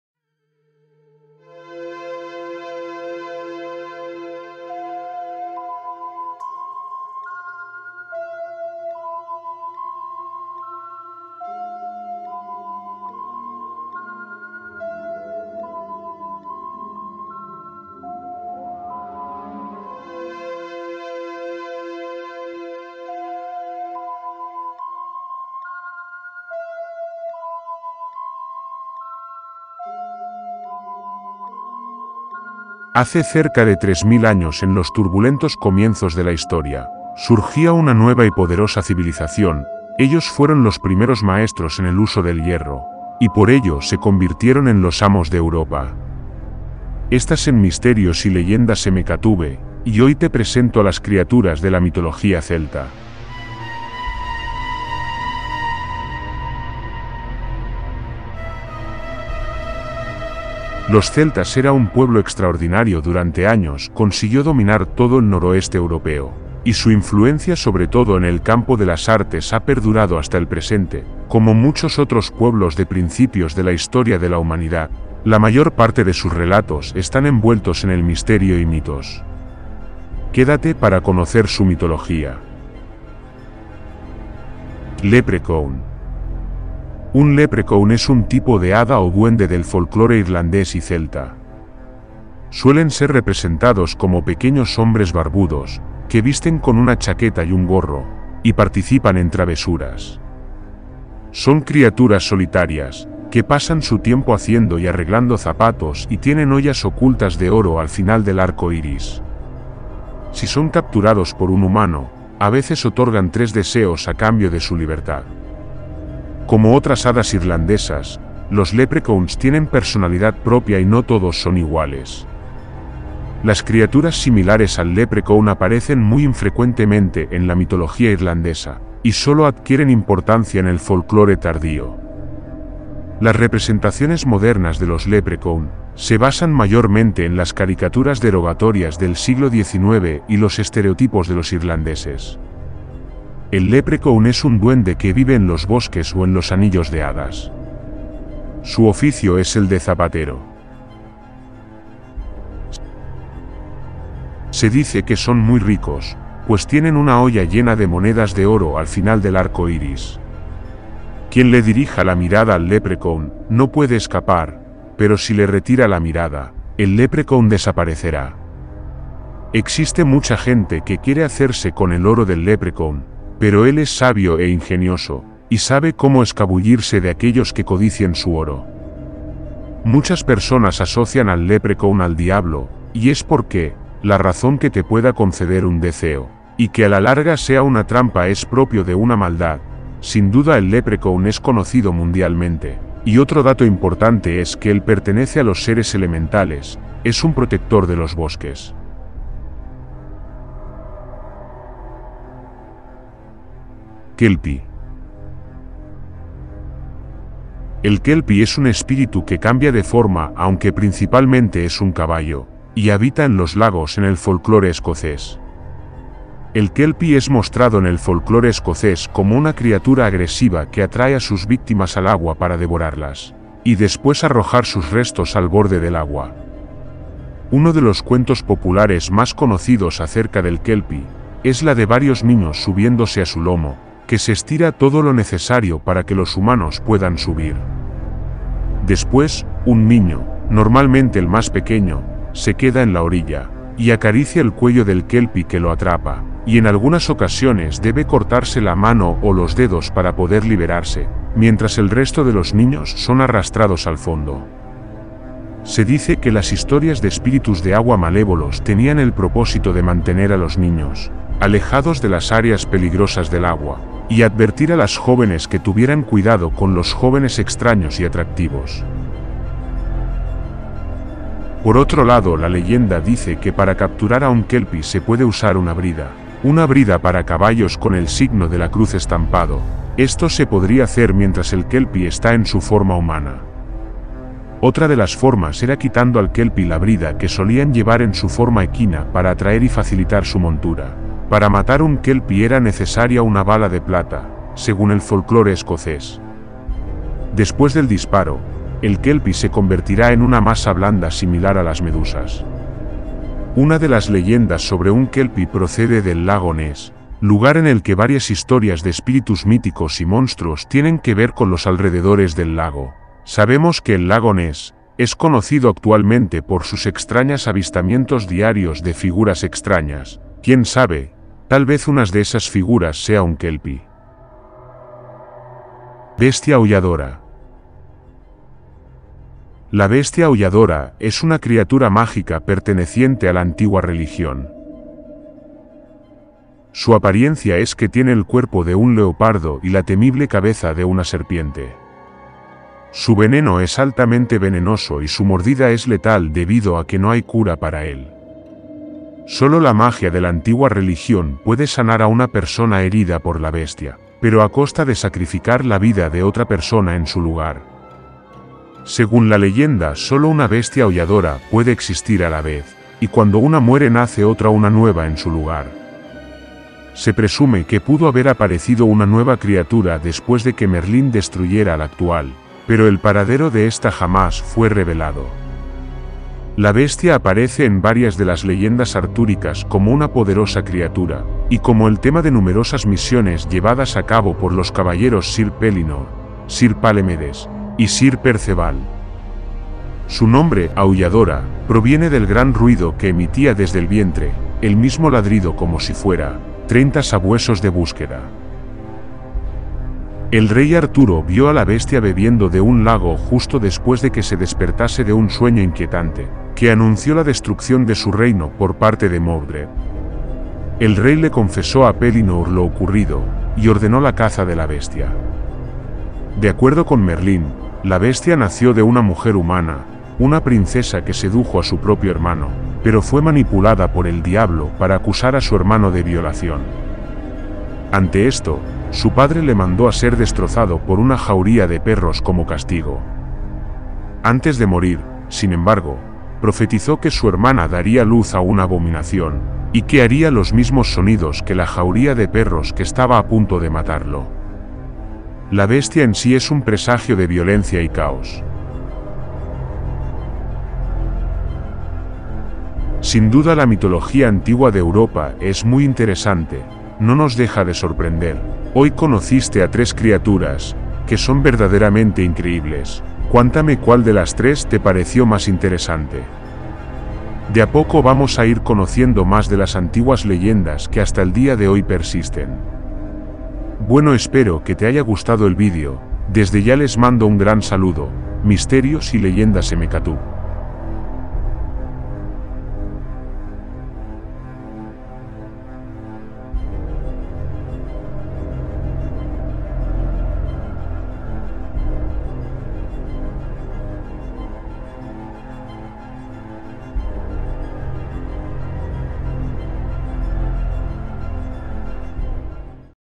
3 Criaturas de la Mitología Celta, Bestiario Mitológico, Part 1
Las Criaturas de la Mitología Celta en un Bestiario Mitológico, Mira la Historia del pasado que hasta hoy persiste en sus historias y es un tema de debate si existieron o no.
Los celtas son un conjunto de pueblos de orígenes diversos que hablaban una lengua llamada celta. La palabra no tiene connotación racial alguna, sino que refiere estrictamente a estructuras socioculturales. La terminología, es además, muy reciente y sirve para clasificar un grupo humano bastante extenso. Los pueblos "celtas" ocuparon a partir del siglo V de la era cristiana, gran parte de Europa, las Islas británicas, el territorio que se extiende desde la desembocadura del Rin hasta los Pirineos y desde el atlántico hasta Bohemia, con prolongaciones en el norte de Italia y Noroeste de España. dale like para apoyar al canal desde ya gracias Misterios y Leyendas MKTUBE.